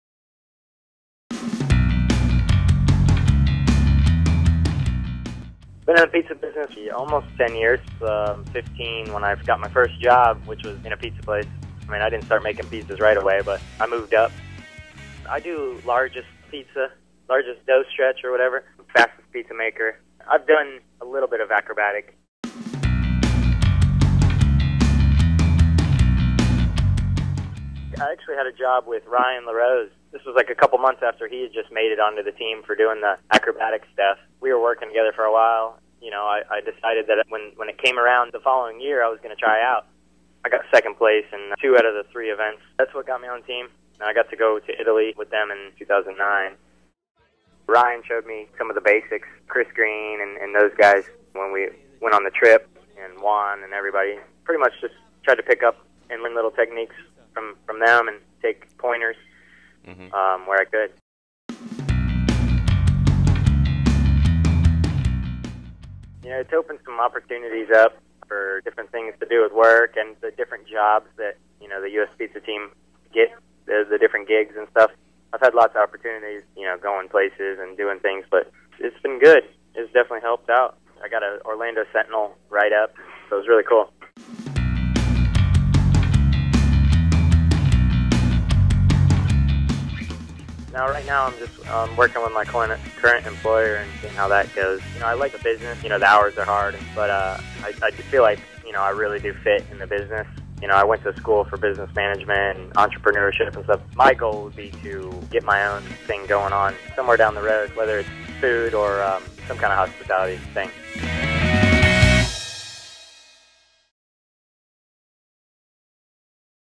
Meet The Team: Dave Moore
View footage of Dave Moore from PMQ's U.S. Pizza Team and pick up the May 2010 issue to find out more.